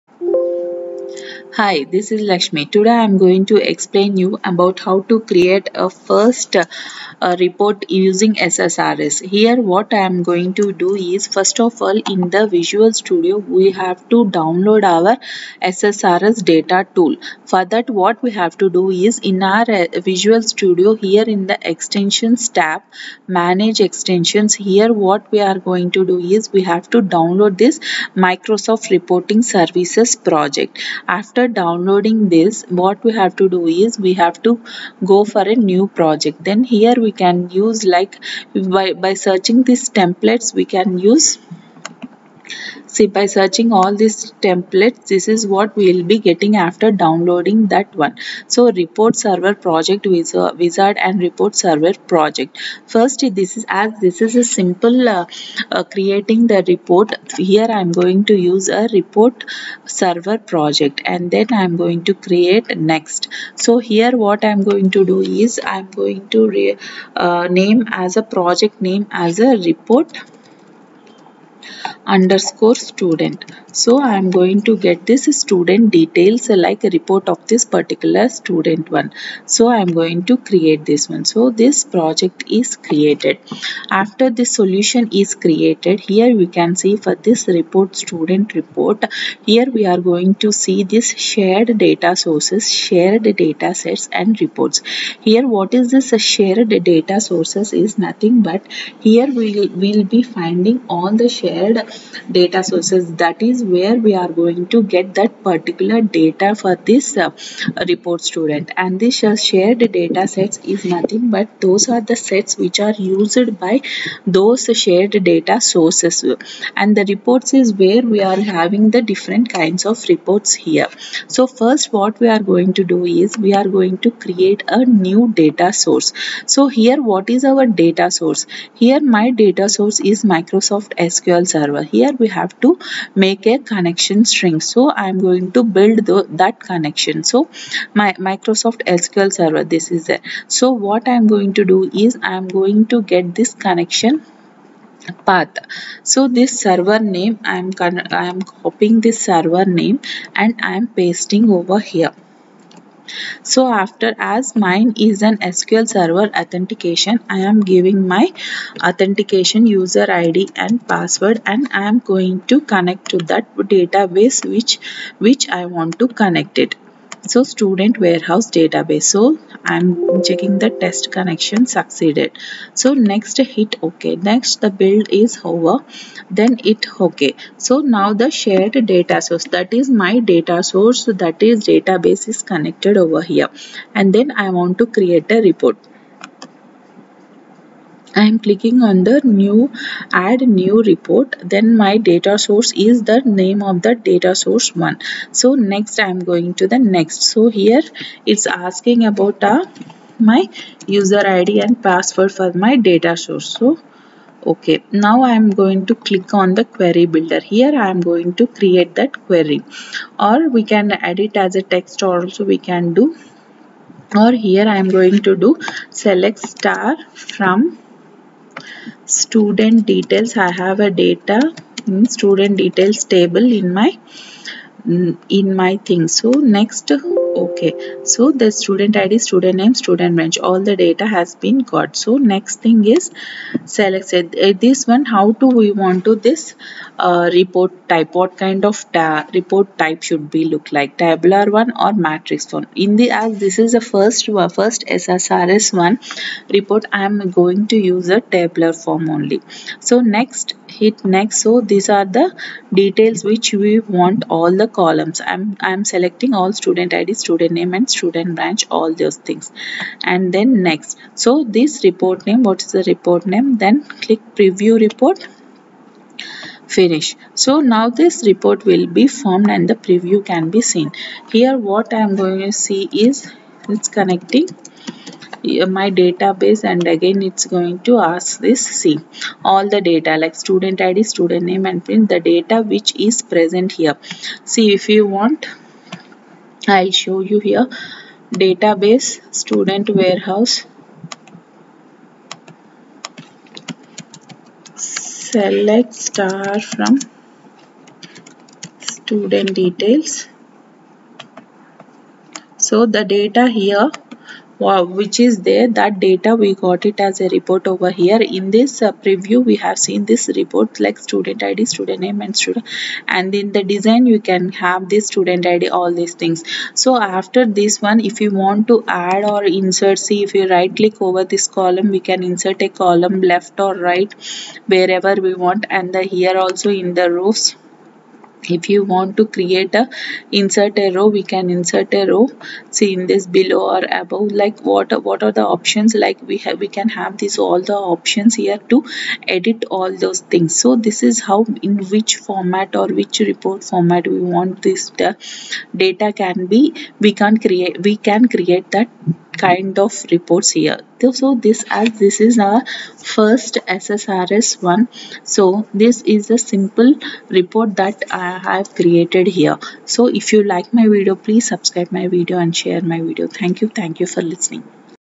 1.SSRS TUTORIAL - CREATING FIRST REPORT
EXPLAINED ABOUT HOW TO CREATE A FIRST SSRS REPORT USING VISUAL STUDIO WITH SIMPLE EXAMPLE .